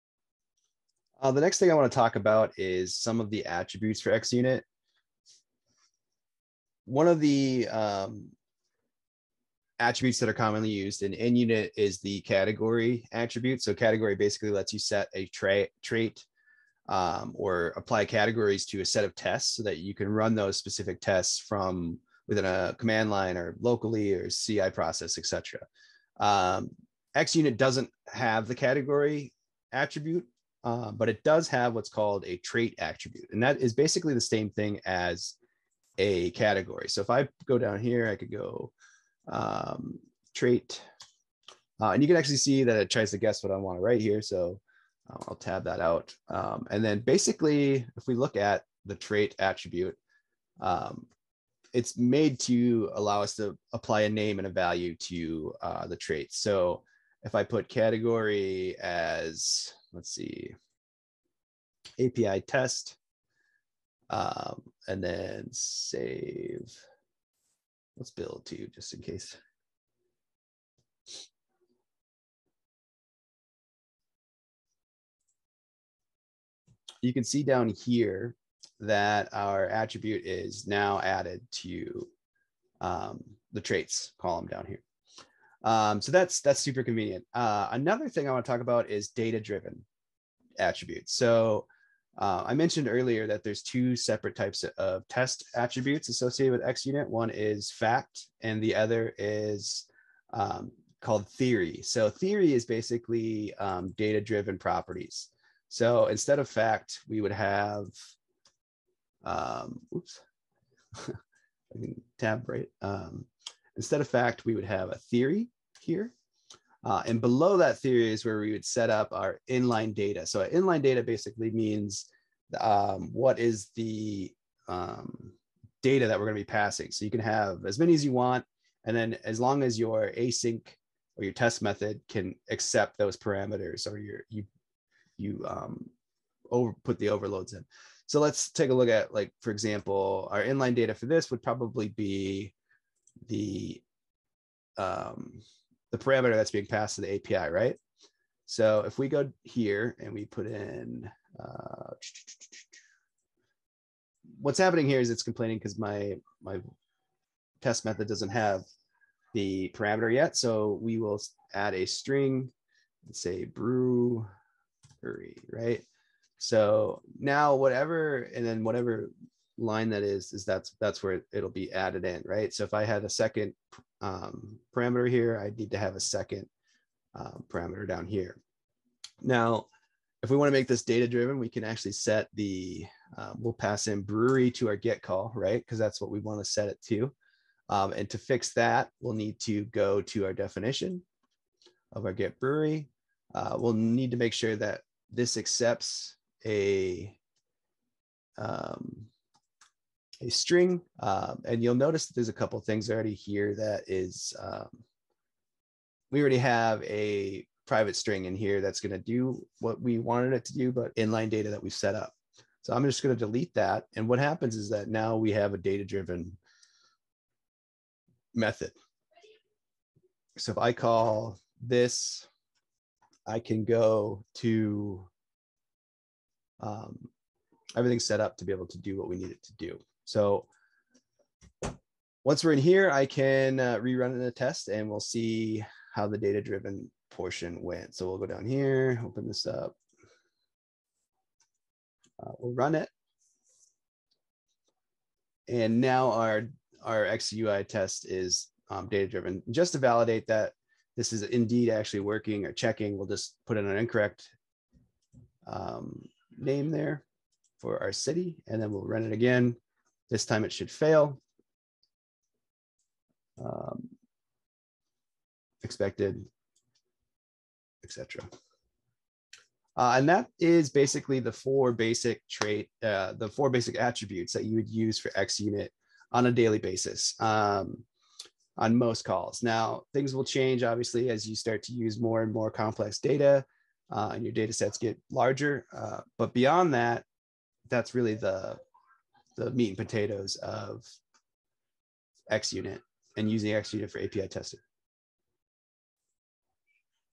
xUnit Attributes: A Comprehensive Guide
In todays session breakout, we are going to go through the most common test attributes used in xUnit testing, FACT, Trait, Theory, and InlineData.